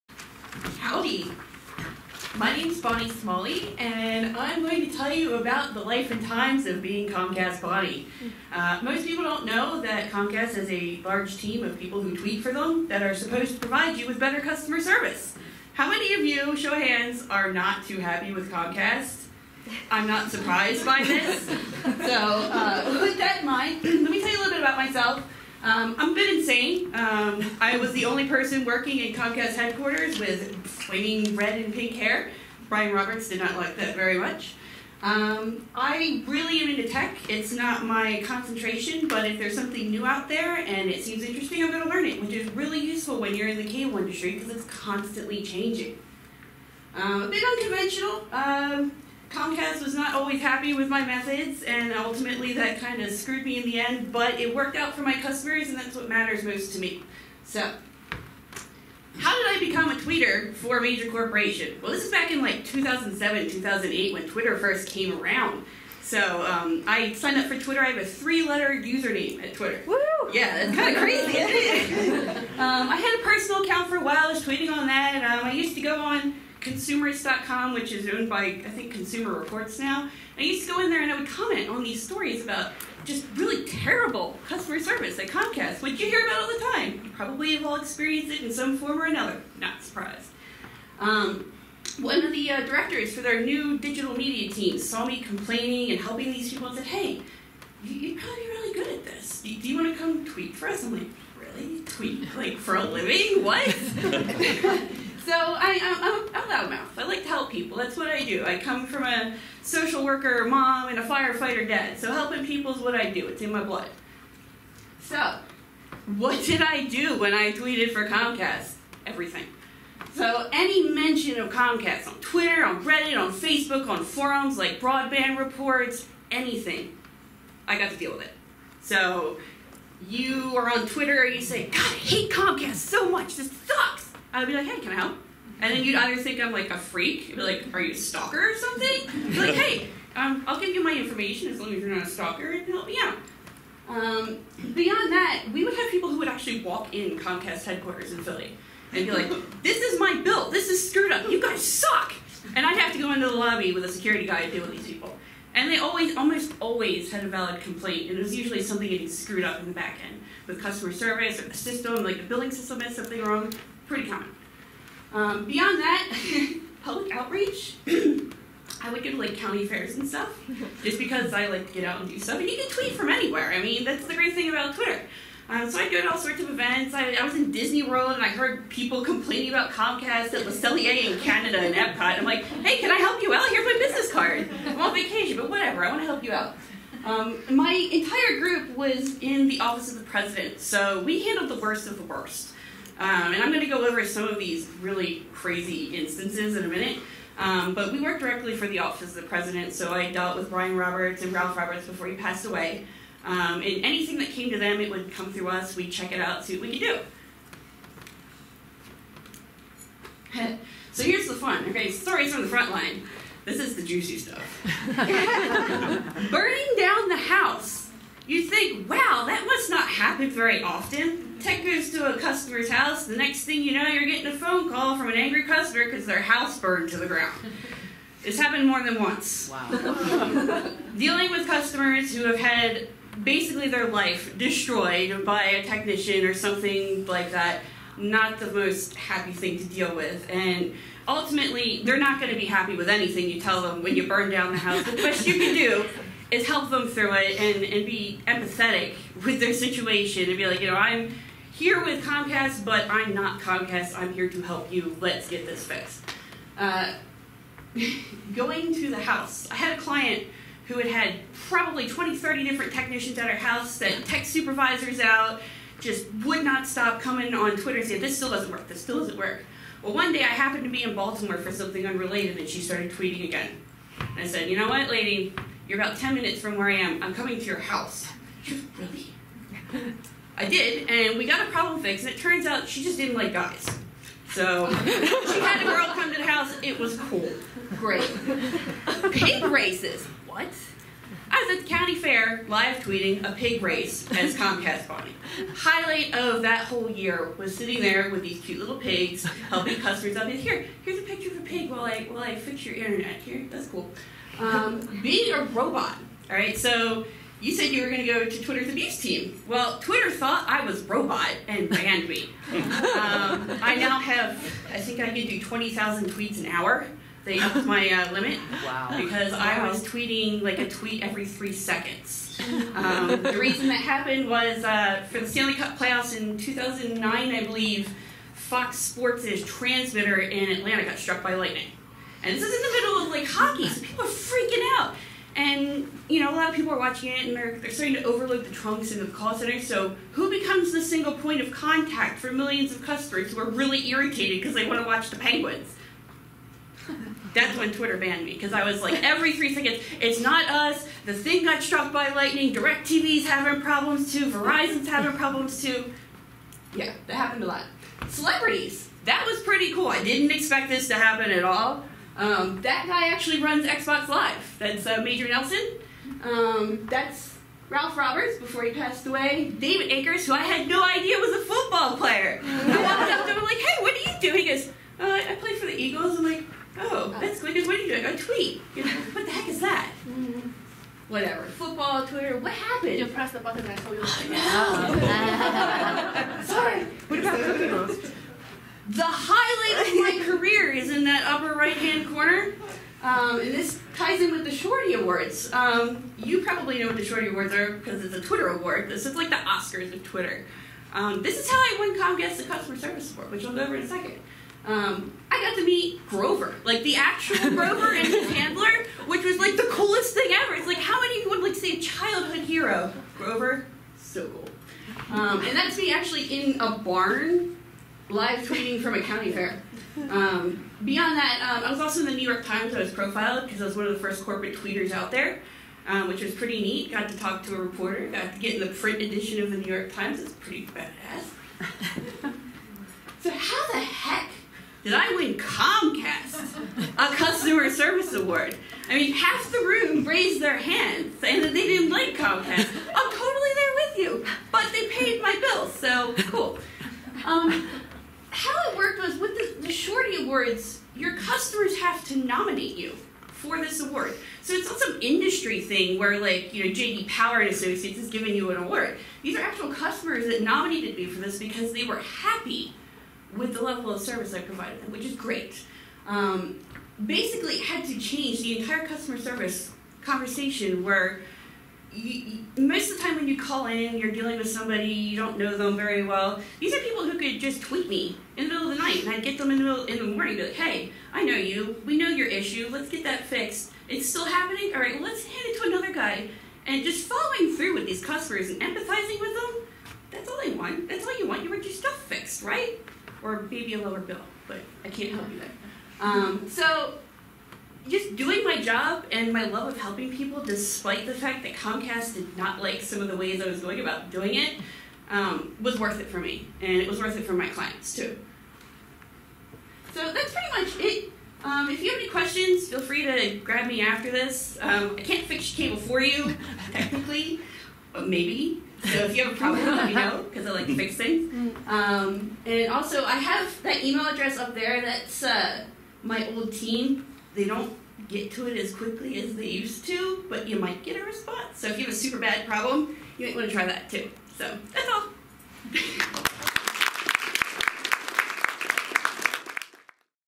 Bonnie Smalley: Stories from the Twitter customer service frontlines
At Women Techmakers by GDG Boulder on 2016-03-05